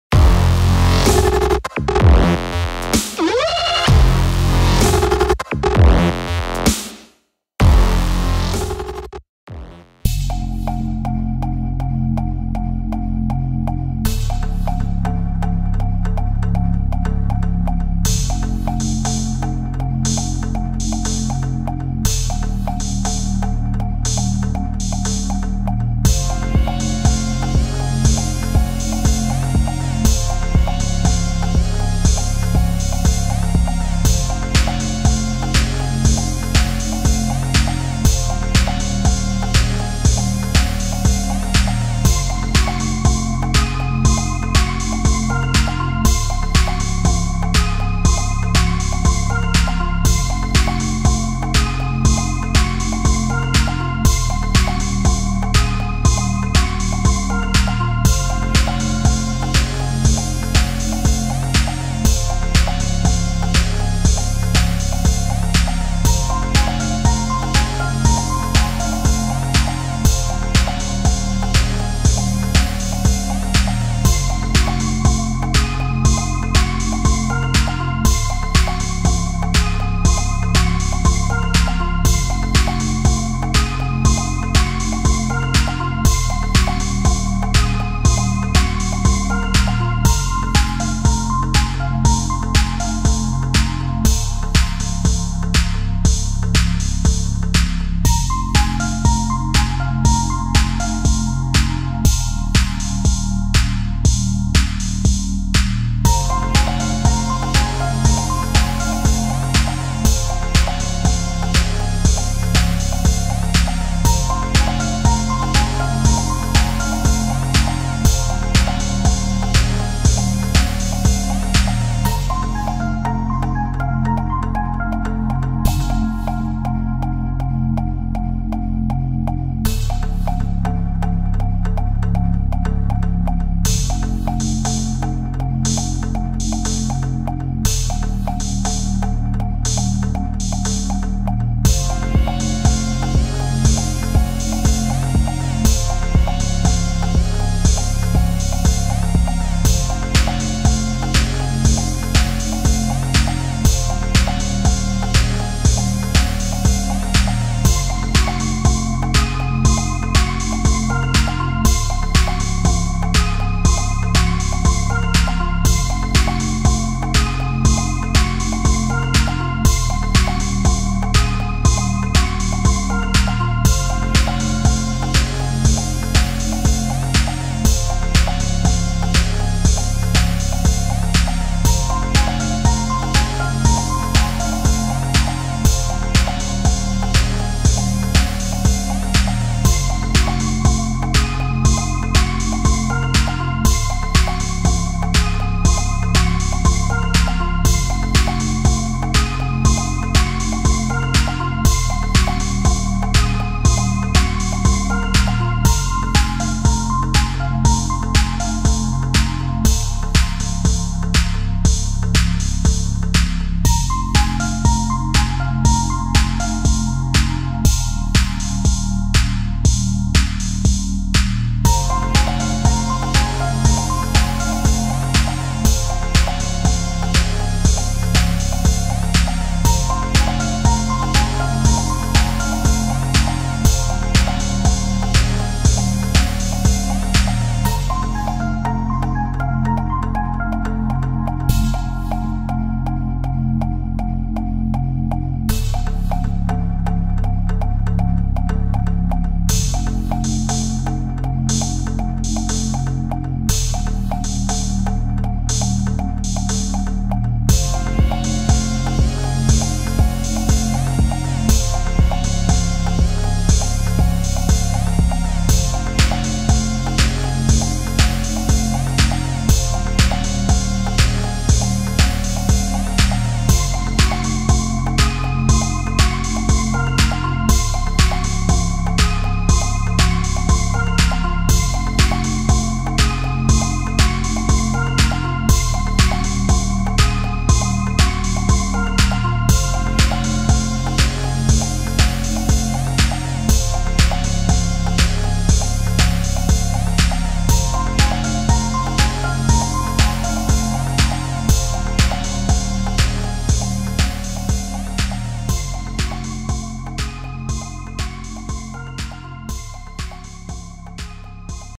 NISSAN NAVARA | Repainting Emblem | Paintwerx Diy
NISSAN NAVARA: Repainting Emblem from chrome finish to glossy black
with red nissan.

Kung wala kayo spray gun at compressor. Pde din kayo gumamit ng spray paint yung na sa lata. Bili lang kayo ng Spray paint na Primer,Black at Clear coat. Lihahin nyo lang maigi ng 360 grit or 400 grit mas gasgas mas makapit. Sa sticker mag pa cut lang kayo sa mga gumagawa ng sticker meron sa mga mall basta dalin nyo lang yung emblem para magaya
yung sukat.

Follow me on:
Visit my Shopee store: PAINTWERX12